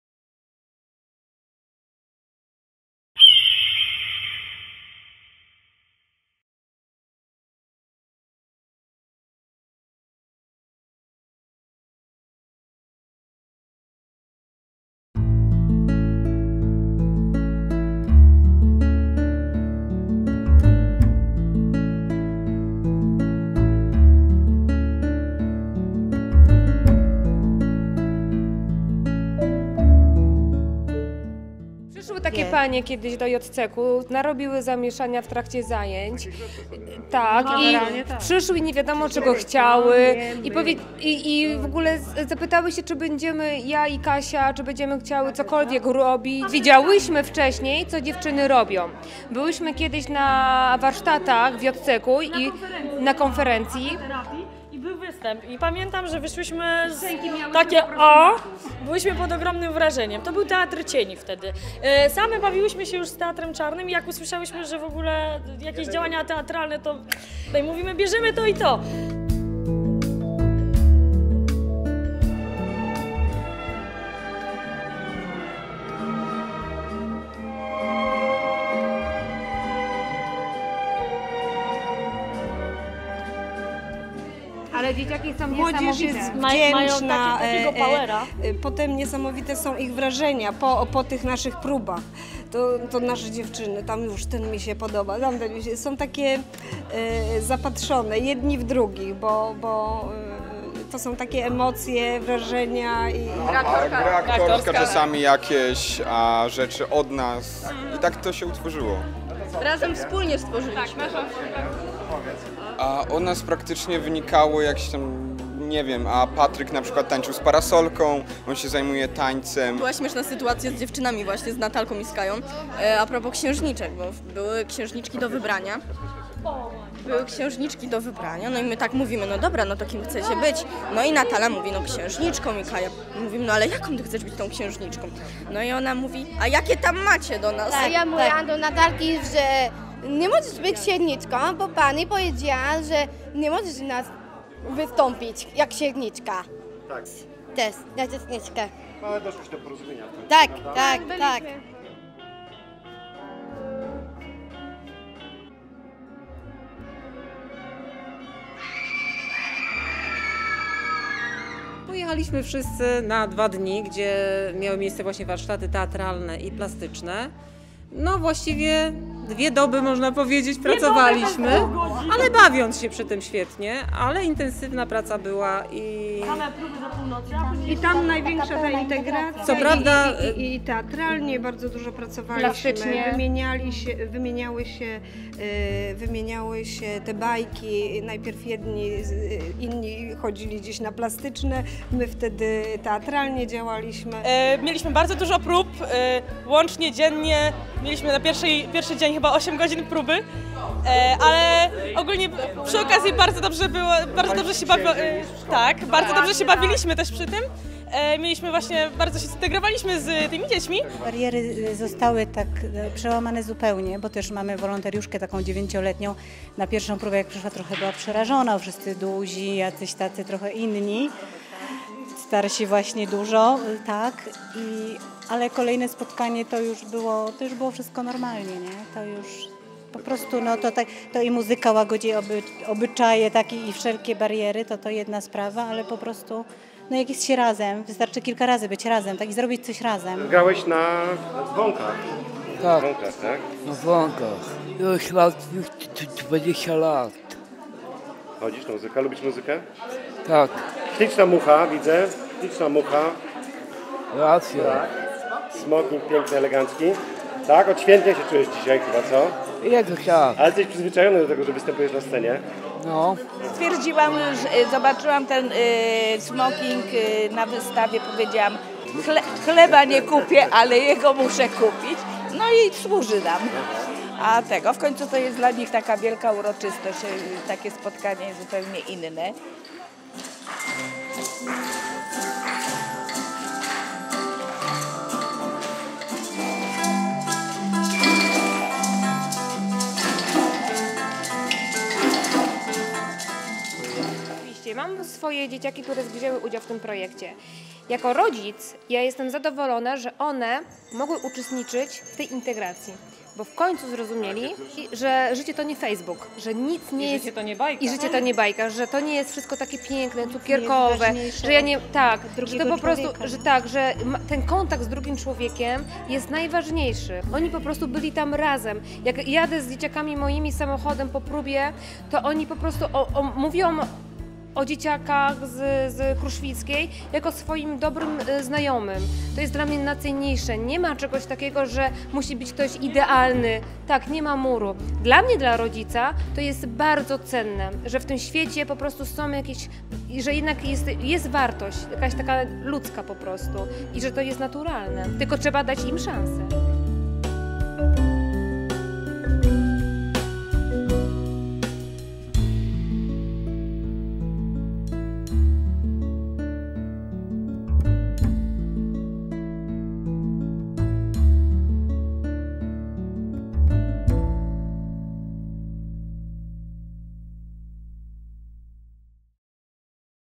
BMK 2017 - Jelenia Góra - "Paczłorkowo" - finał projektu BMK
„Paczłorkowo” to połączenie różnych dziedzin sztuki, różnorodnych działań twórczych oraz różnych środowisk, odwołujących się do uniwersalnego język teatru, muzyki i plastyki, zakończone spektaklem wystawionym na scenie jednego z teatrów jeleniogórskich. Scenariusz, scenografia i muzyka stworzone będą przez jeleniogórską młodzież gimnazjalną, uczniów z niepełnosprawnością intelektualną ze Specjalnego Ośrodka Szkolno-Wychowawczego, uczestników Muzycznej Grupy Integracyjnej PENTATONIKA. Projekt będzie realizowany od 11 września do 21 października. W skład zespołu projektowego wchodzą: Katarzyna Mokanek – Piątek, Edyta Chwalibóg, Beata Ignatowska.